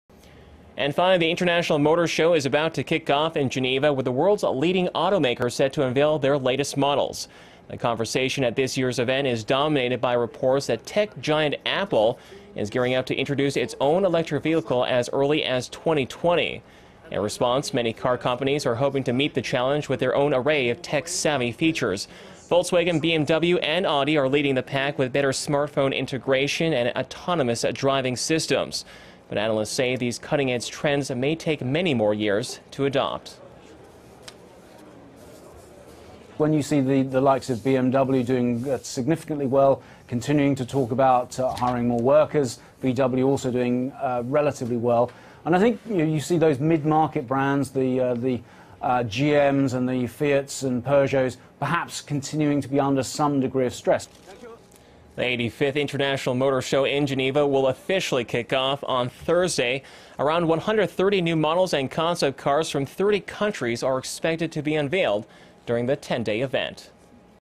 Tech challenges raised at Geneva motor show 2015 제네바 모터쇼 개막
And finally, the International Motor Show is about to kick off in Geneva... with the world′s leading automakers set to unveil their latest models.
The conversation at this year′s event is being dominated by reports that tech giant Apple... is gearing up to introduce its own electric vehicle as early as 2020.
In response, many car companies are hoping to meet the challenge with their own array of tech-savvy features.
Volkswagen, BMW, and Audi are leading the pack with better smartphone integration... and autonomous driving systems.
But analysts say these cutting-edge trends may take many more years to adopt.

"When you see the likes of BMW doing significantly well. Continuing to talk about hiring more workers, VW also doing relatively well and I think you see those mid-market brands, the GM′s and the Fiats and Peugeots perhaps continuing to be under some degree of stress."
The 85th International Motor Show in Geneva... will officially kick off on Thursday.
Around 1-hundred-30 new models and concept cars from 30 countries are expected to be unveiled during the 10-day event.
And that wraps up our look at international stories for now. I′ll see you back here tomorrow night.